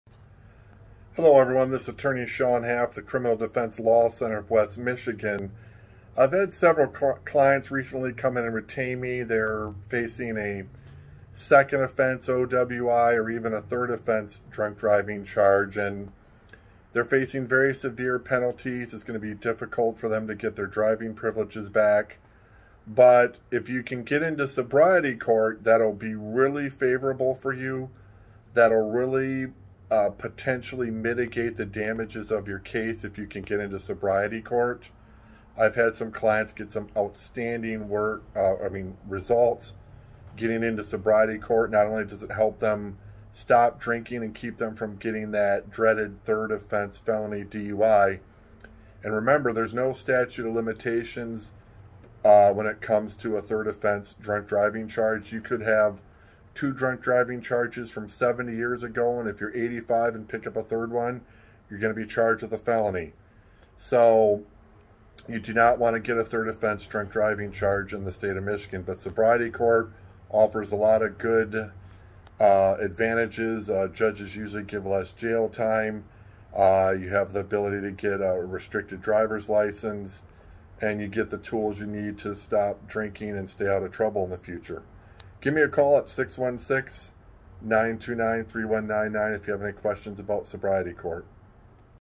Sobriety Court in West Michigan
Sobriety Court is an important options for those who are facing second offense DUI's in Grand Rapids, Michigan or felony drunk driving charges in Grand Rapids, Michigan.